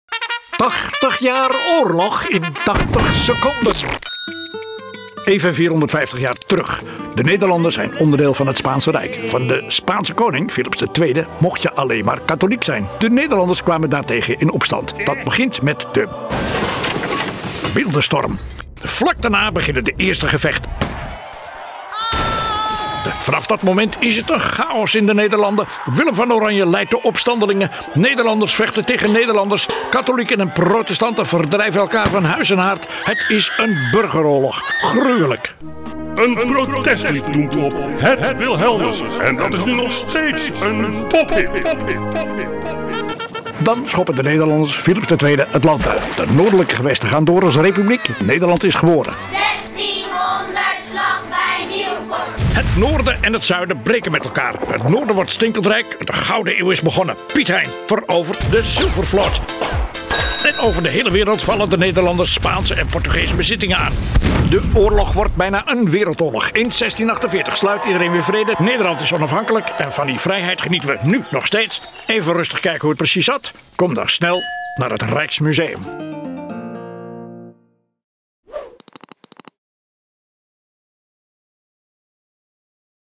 Rijksmuseum - 80 Jaar Oorlog In 80 Seconden
‘80 jaar oorlog’, van Gijs van der Ham en met bijdragen van anderen is een boek gebaseerd op verhalen uit de gelijknamige NTR-serie en de tentoonstelling in het Rijksmuseum, en vertelt het verloop van de oorlog aan de hand van tijdloze thema’s, objecten die een rol speelden in het conflict, kunstwerken en persoonlijke getuigenissen.

Met een eigentijds tentoonstellingsontwerp van de Vlaamse theatervormgever Roel van Berckelaer toont het Rijksmuseum hoe de Tachtigjarige Oorlog Nederland heeft veranderd en gevormd. Ook wordt duidelijk hoe het zuiden, nu België, door deze strijd een eigen karakter kreeg. 80 Jaar Oorlog. De geboorte van Nederland is de eerste grote tentoonstelling die het gehele conflict belicht en ook het internationale perspectief ervan laat zien. Kwesties als godsdienstvrijheid, zelfbeschikkingsrecht, terreur en vervolging die ook vandaag de dag actueel zijn, duiken steeds op.